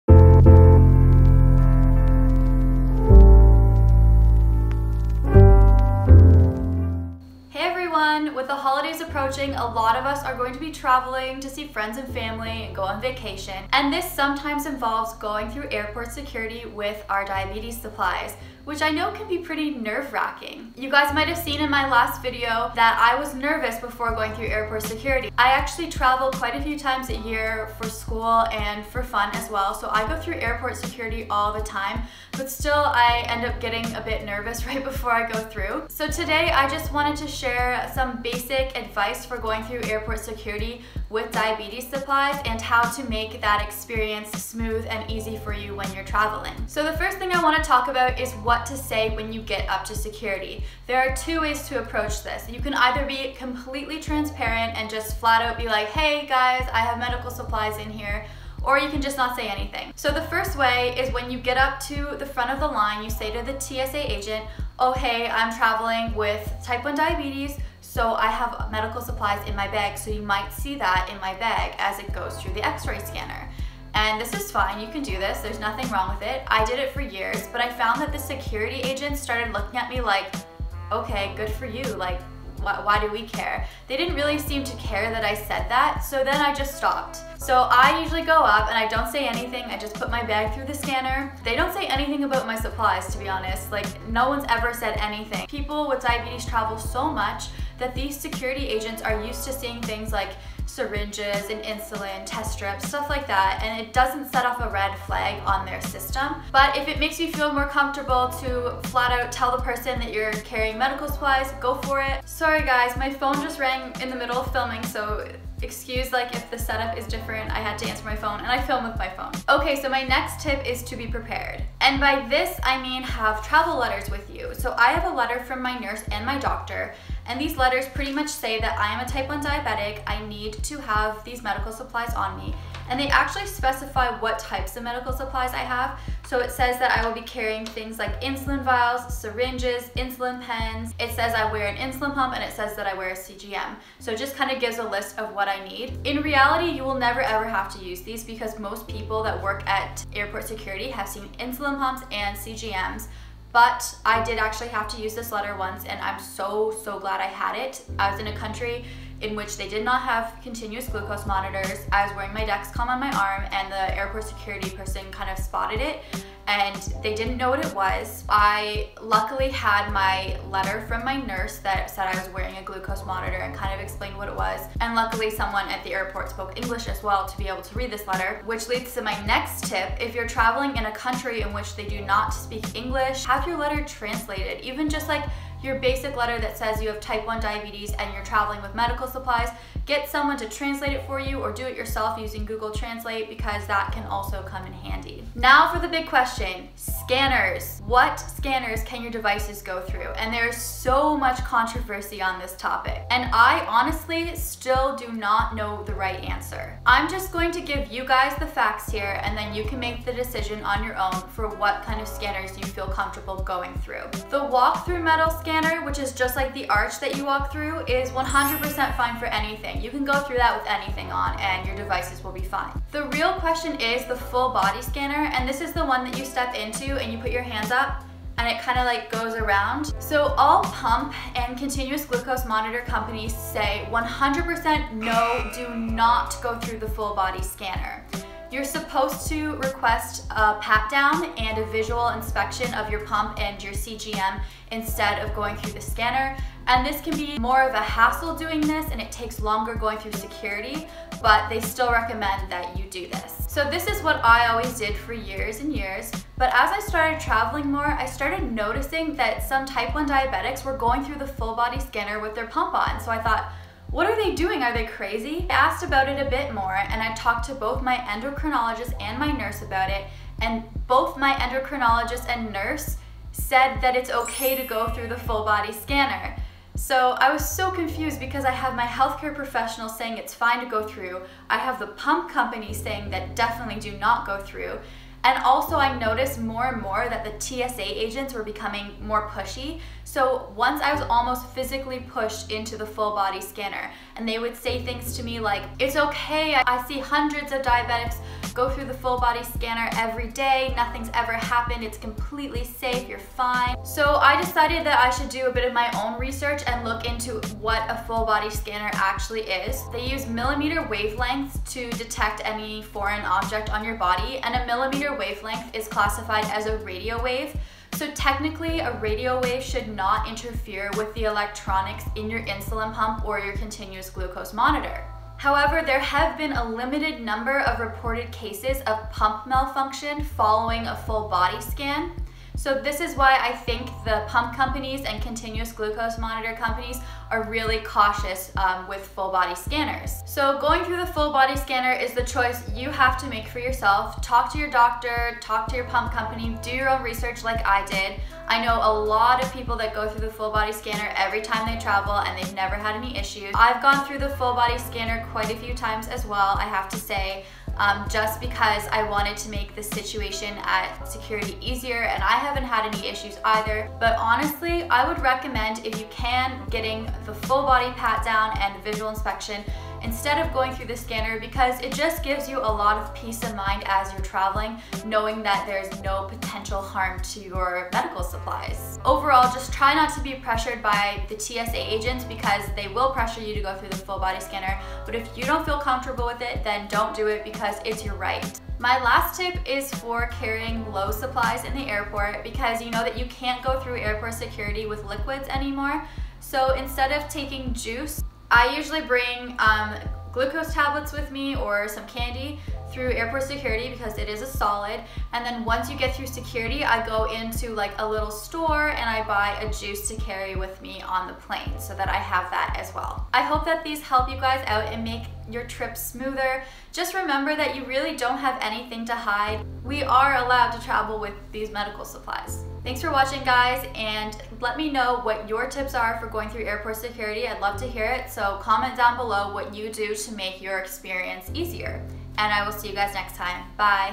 AIRPORT SECURITY WITH DIABETES SUPPLIES
Hey everyone! I wanted to give some basic tips for how to go through airport security with diabetes supplies. I've always struggled with the question "Can you go through a full body scanner with an insulin pump?" (3:43), and I've been given conflicting information regarding this. Going through a full body scanner with an insulin pump is something you will have to decide for yourself, so make sure to do your own research and talk to your pump company/doctor before flying :). Happy Travels!